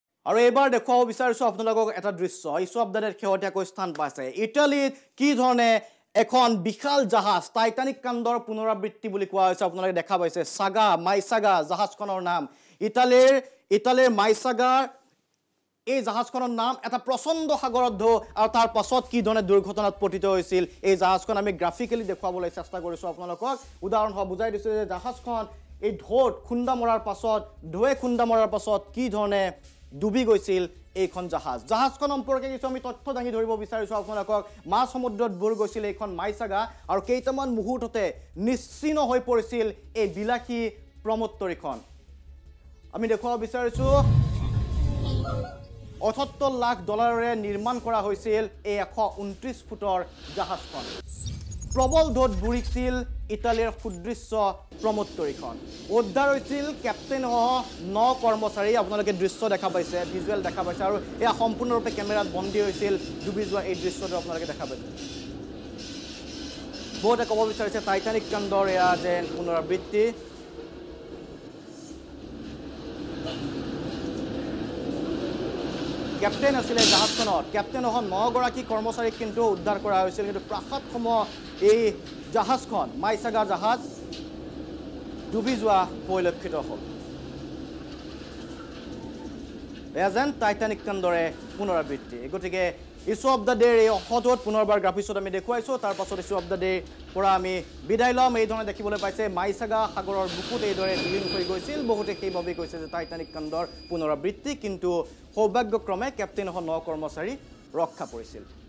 Dramatic video captures the moment a massive 40-meter superyacht sank off the coast of Italy
Dramatic video captures the moment a massive 40-meter superyacht sank off the coast of Italy Watch the full clip to know more about this story.